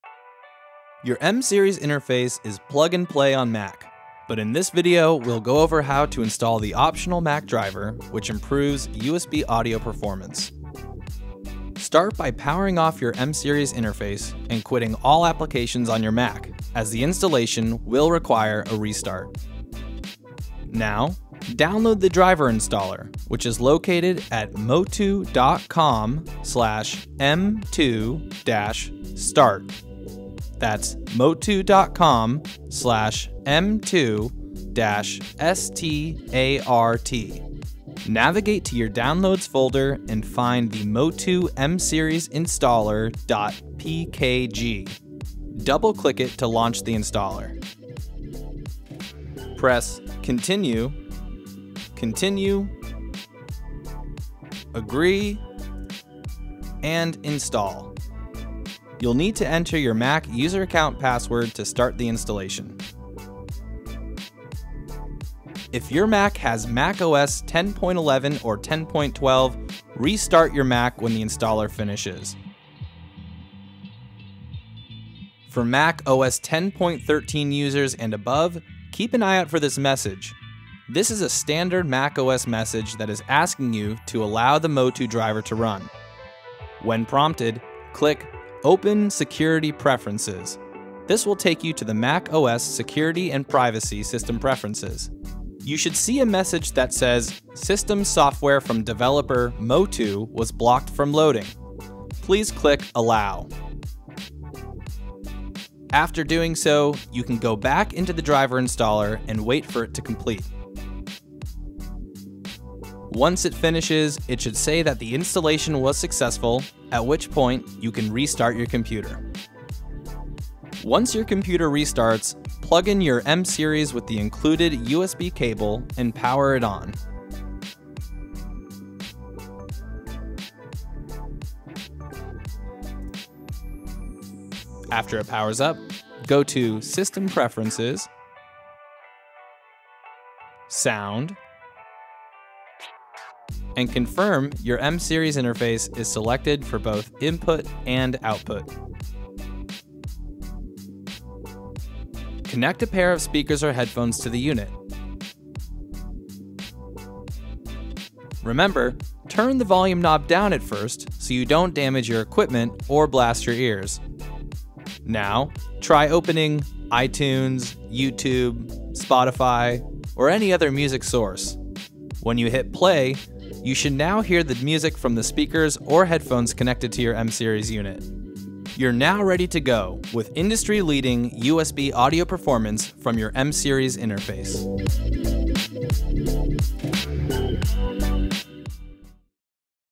M series optional Mac driver installation
Your M series interface is plug-and-play with a Mac: no driver required. However, the optional Mac driver covered in this video gives you the best possible USB speed (USB latency performance) from your host audio software, plus convenient "loopback" channels that let you easily capture your computer's output. Combine computer audio with live mics and/or guitar inputs to stream the combined mix to Youtube or your favorite podcasting platform in real time. Streaming and podcasting have never been easier and better-sounding!